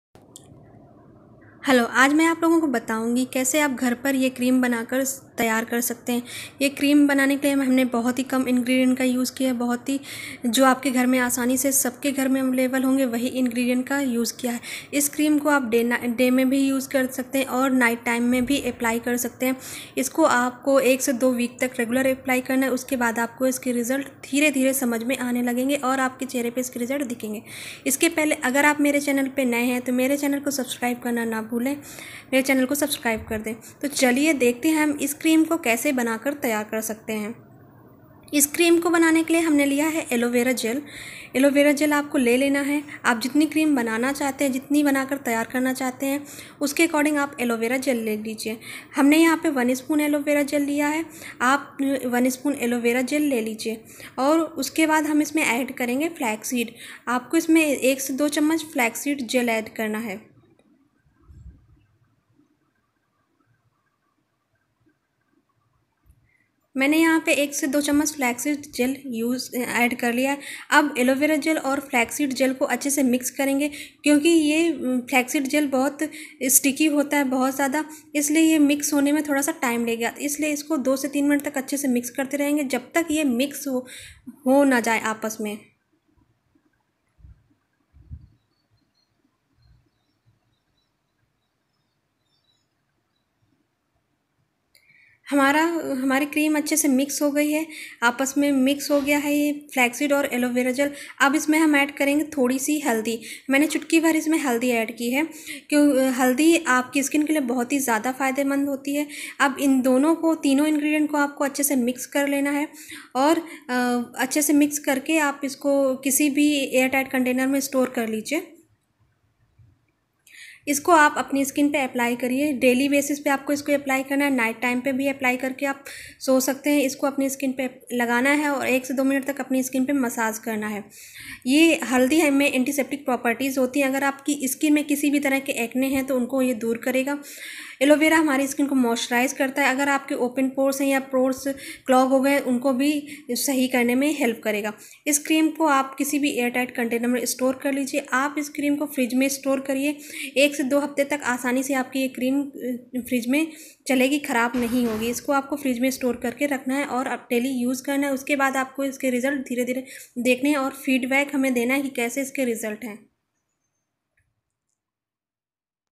Apply this Miracle Gel Cream Daily And Get Glowing Spotless Skin | Glass Skin like Koreans
how to make this cream at home very simple cream only from three ingredients

Ingredients

Alovera gel
flexseeds gel
Turmeric

How to get glass skin like Koreans
miracle skin whitening cream
glowing skin at home
apply this cream and get fair skin